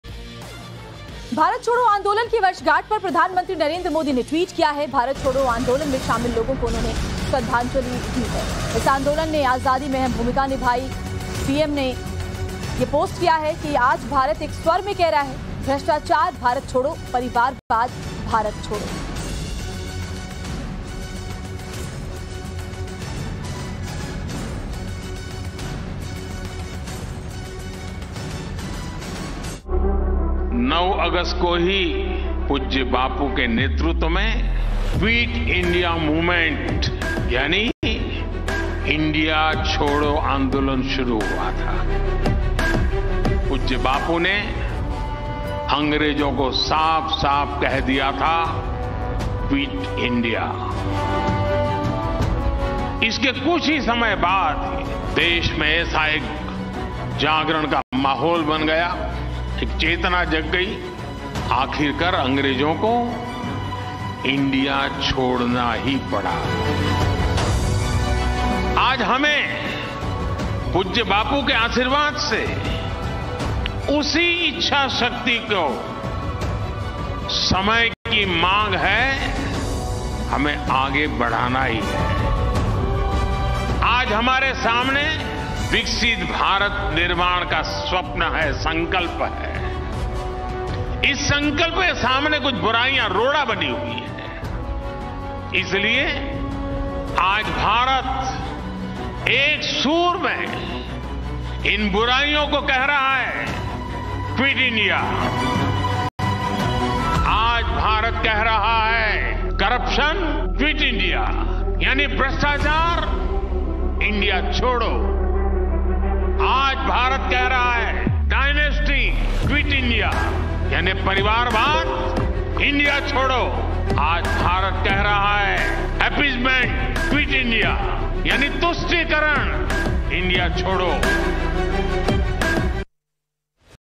भारत छोड़ो आंदोलन की वर्षगांठ पर PM Modi का Tweet, 'भ्रष्टाचार भारत छोड़ो, परिवारवाद...' | Hindi News
भारत छोड़ो आंदोलन की वर्षगांठ पर PM Modi ने ट्वीट किया है .पीएम मोदी ने अपने ट्वीट में कहा, 'आज भारत एक स्वर में कह रहा है, भ्रष्टाचार भारत छोड़ो, परिवारवाद भारत छोड़ो'